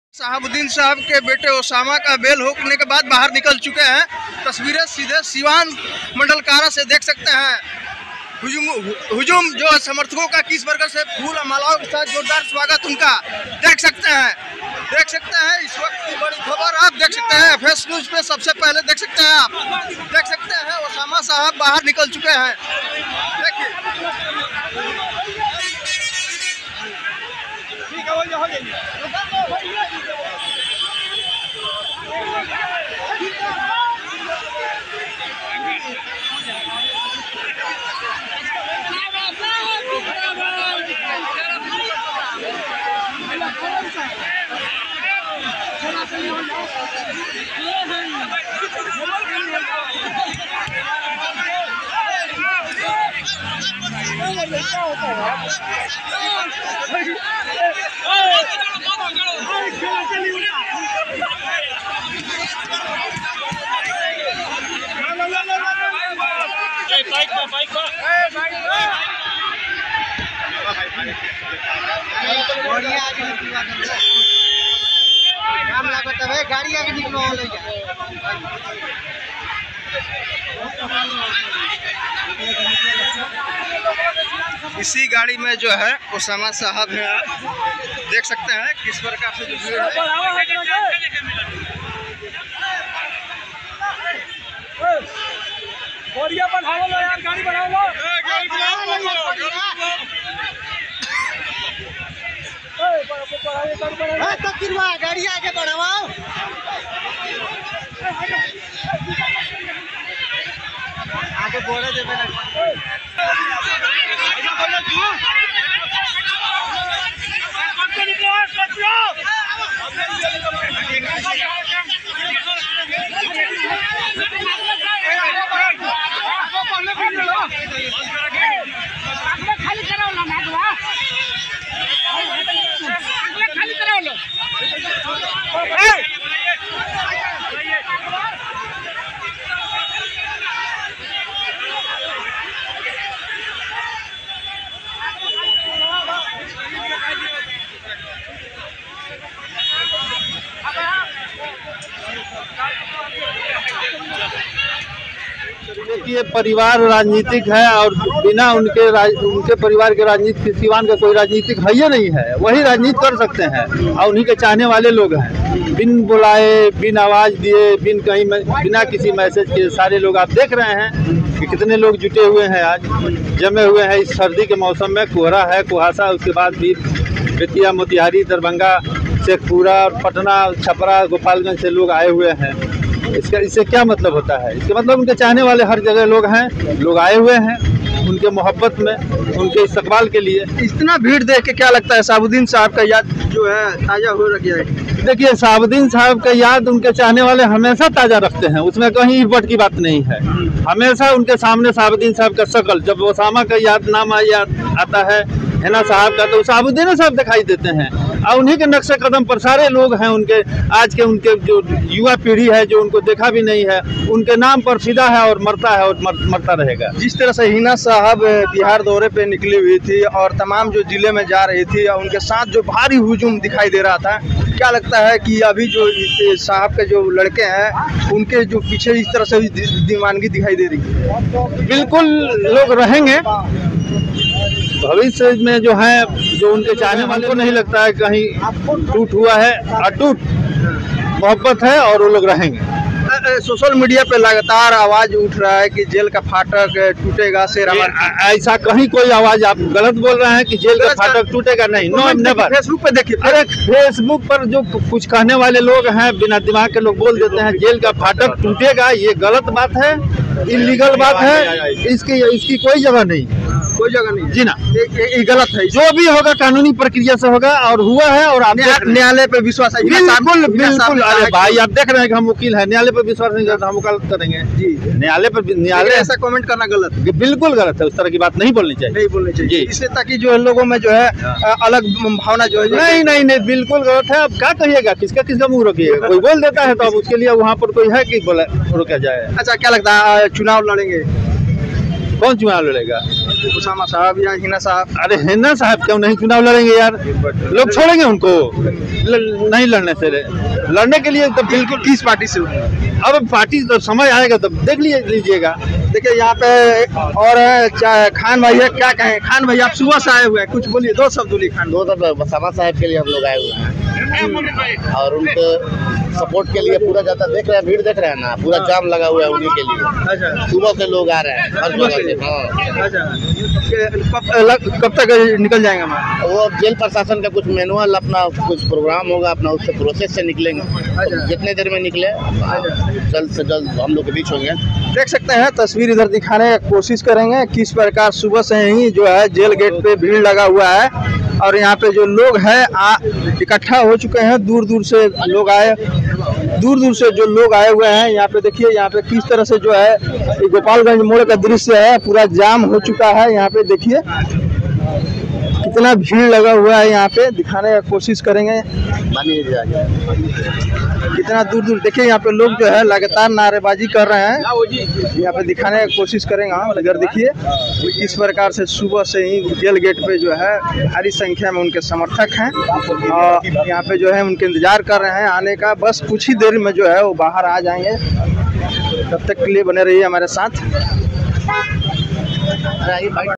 शहाबुद्दीन के बेटे ओसामा निकले जेल से शेर आया शेर के लगे नारे@FSNEWSTimes
शहाबुद्दीन के बेटे ओसामा निकले जेल से शेर आया शेर के लगे नारे